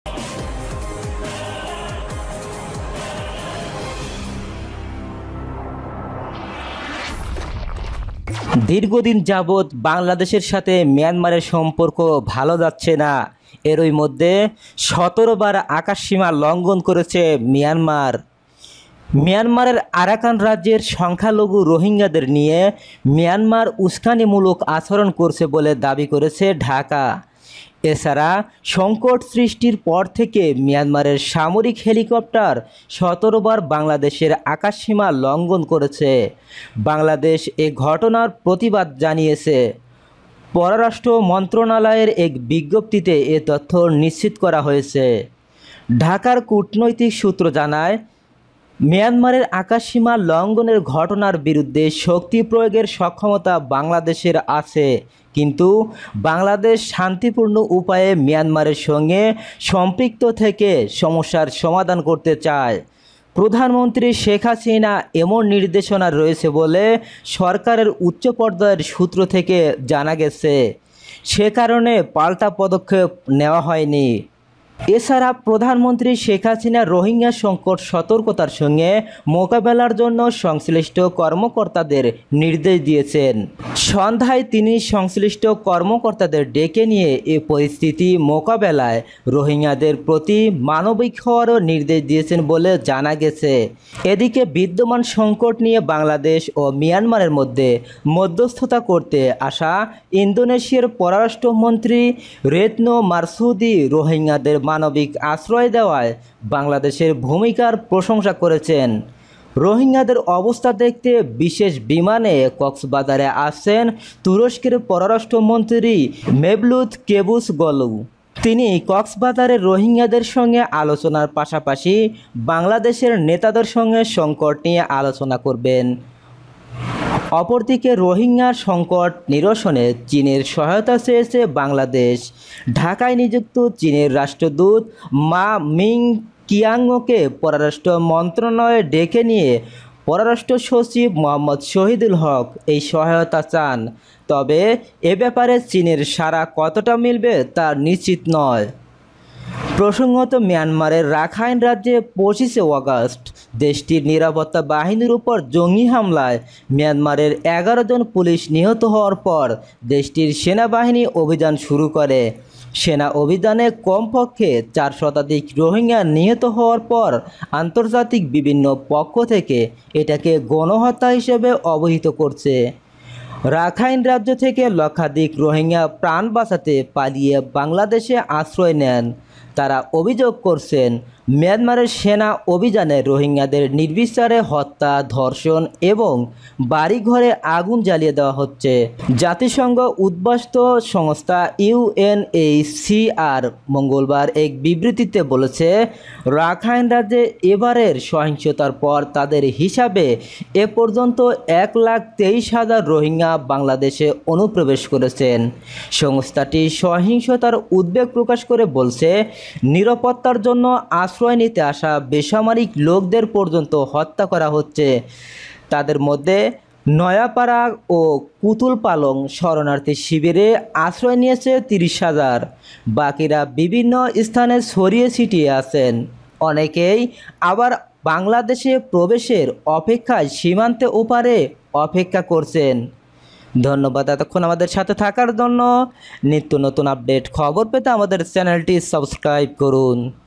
১৭ বার বাংলাদেশর আকাশসীমা লঙ্ঘন করেছে মিয়ানমার-তাহলে কি যুদ্ধ বেধেই গেল ?? কি কি পদক্ষেপ নিচ্ছে
বাংলাদেশ এ ঘটনার প্রতিবাদ জানিয়েছে। পররাষ্ট্র মন্ত্রণালয়ের এক বিজ্ঞপ্তিতে এ তথ্য নিশ্চিত করা হয়েছে। ঢাকার কূটনৈতিক সূত্র জানায়, মিয়ানমারের আকাশসীমা লঙ্ঘনের ঘটনার বিরুদ্ধে শক্তি প্রয়োগের সক্ষমতা বাংলাদেশের আছে।
---ভিডিওটি facebook এ শেয়ার করে সবাইকে দেখার সুযোগ করে দিন